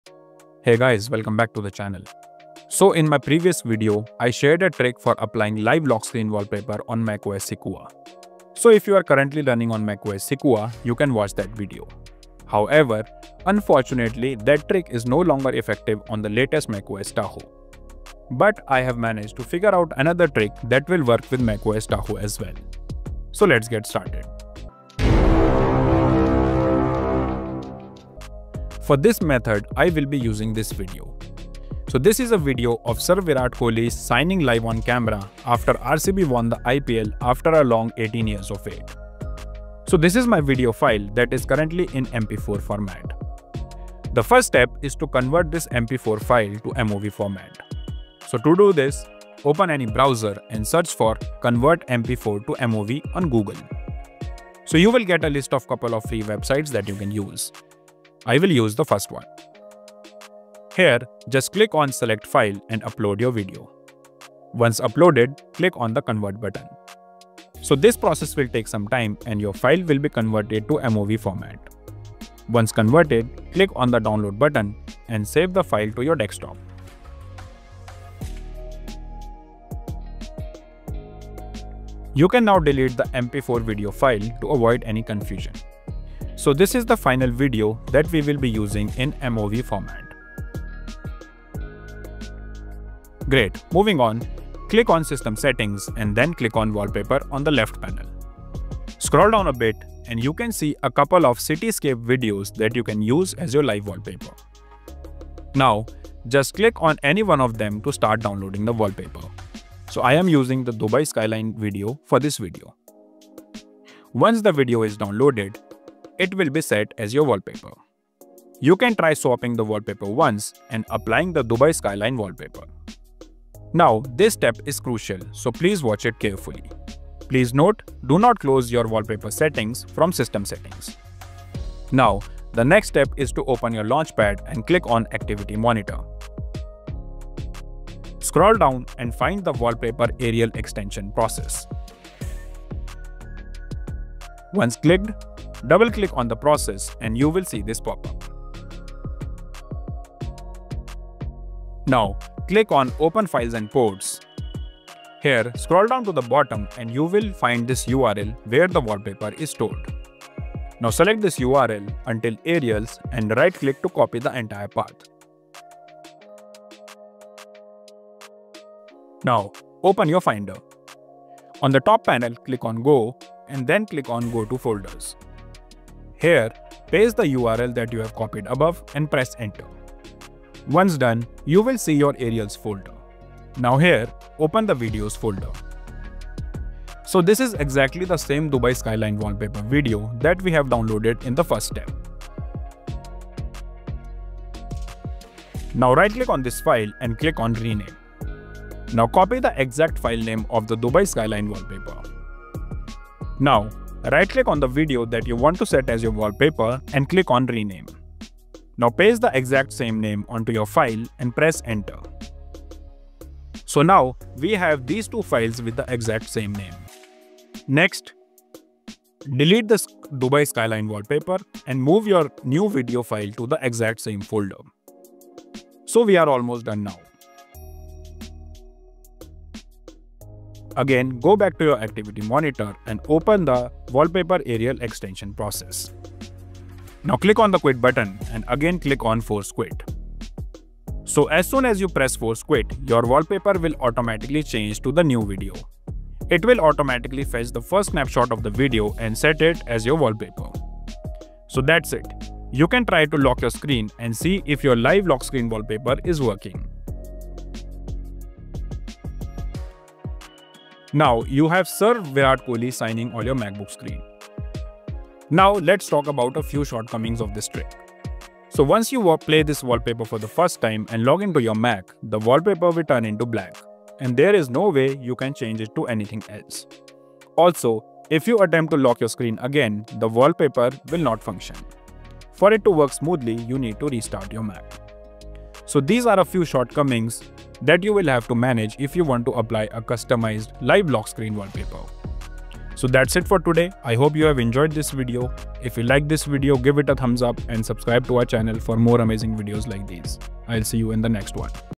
Get ANY Video as Your Live Lock Screen Wallpaper on macOS Tahoe!
Set any video as your live lock screen wallpaper on the latest macOS Tahoe! This simple trick lets you personalize your Mac with clips, such as Virat Kohli's iconic IPL signing moment, without needing any special software.

⏰ TIMESTAMPS / CHAPTERS:
0:00 - Intro
0:28 - What You Need
0:43 - Convert MP4 to MOV
1:32 - Download Apple's Wallpaper
2:03 - The Crucial Step: Find Wallpaper Folder
3:15 - Replace the Video File
3:50 - Activate Your New Wallpaper
4:24 - Important Shortcomings & Fixes

⚠️ NOTE: This method has a known issue where the desktop wallpaper may turn black after login. A simple restart is required to restore the live lock screen functionality. This is a workaround, not a perfect solution, but it's the only one that works on macOS Tahoe at the moment.